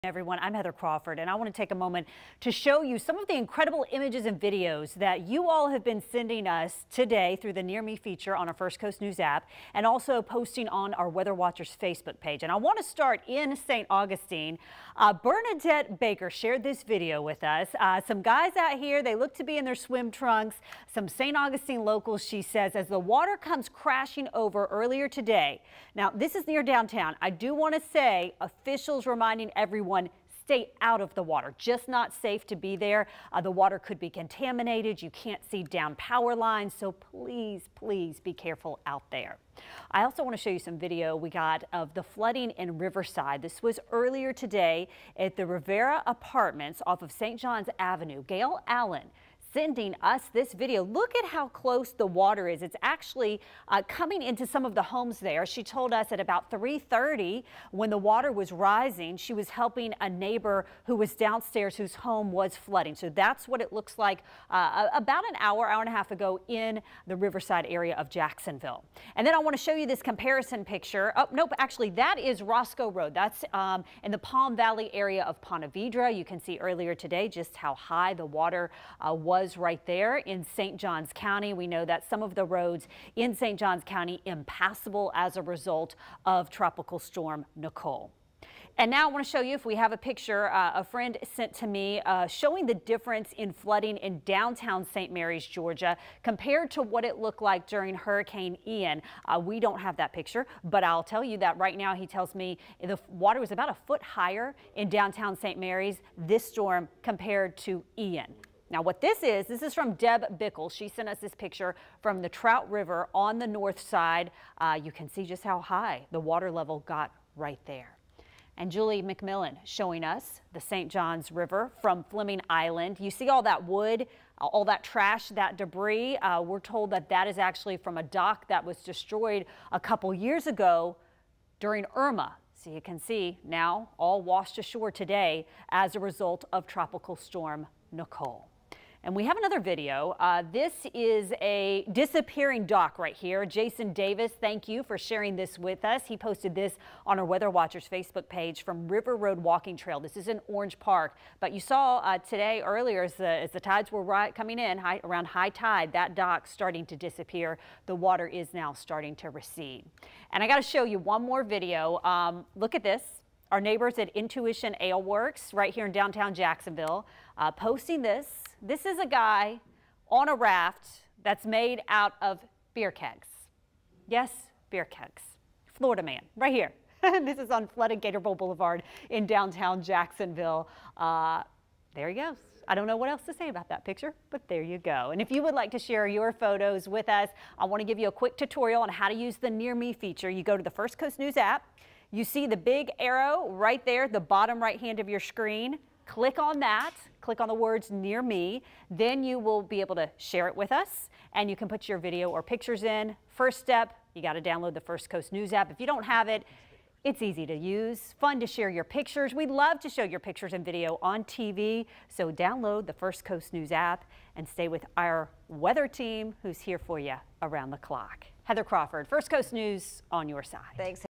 Florida Man sails down floodwaters in beer keg raft | Viewers help report on Tropical Storm Nicole
We're sharing some incredible photos and videos viewers have submitted to us from the storm as it passes through the First Coast.